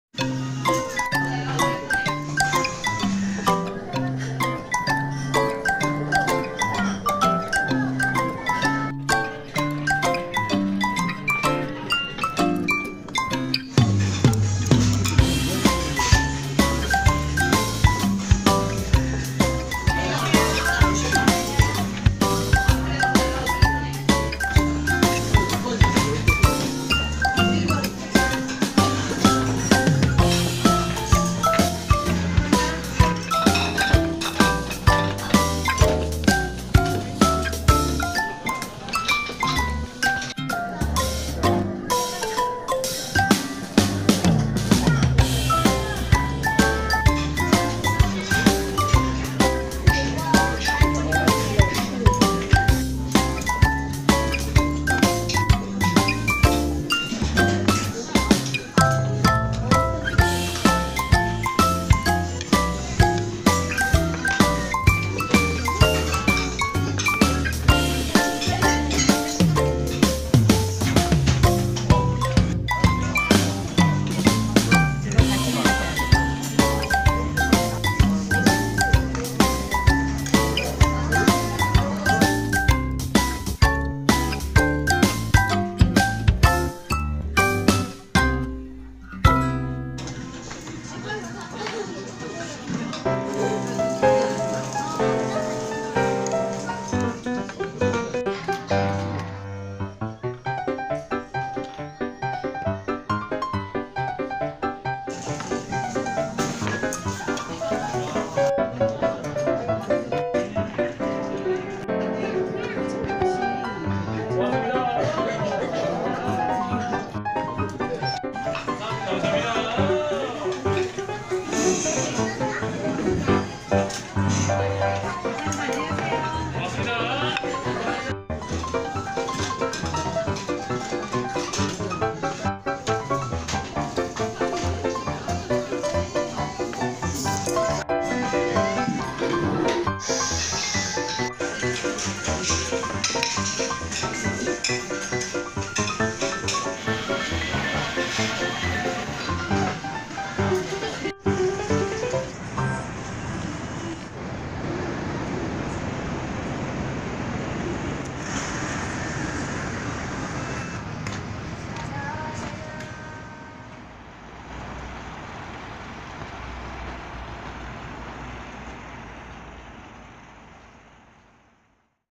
(맛집)/회전초밥/🍣초밥/ 일식집/민락2지구 맛집
*의정부 맛집
*문영부 스시 회전초밥 전문점/의정부 민락2지구
*한접시에  균일가격 1690원

미소된장국 굴 연어회 새우회 푸드 food 레스토랑 restaurant